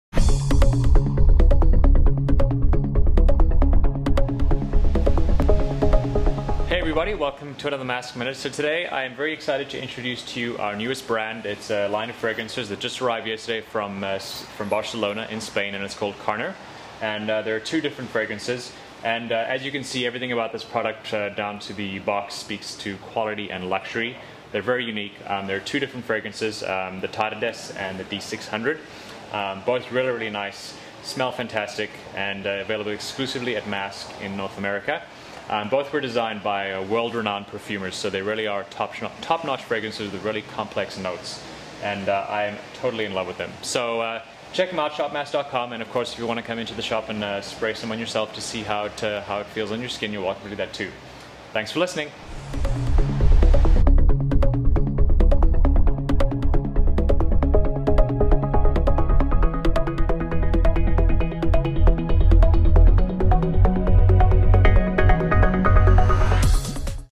NEW Fragrance For Men: Carner Barcelona
In this Masc Minute, Jamie talks about two of Carner Barcelona's fragrances: D600 and Tardes. Exclusively available at shopmasc.com in North America.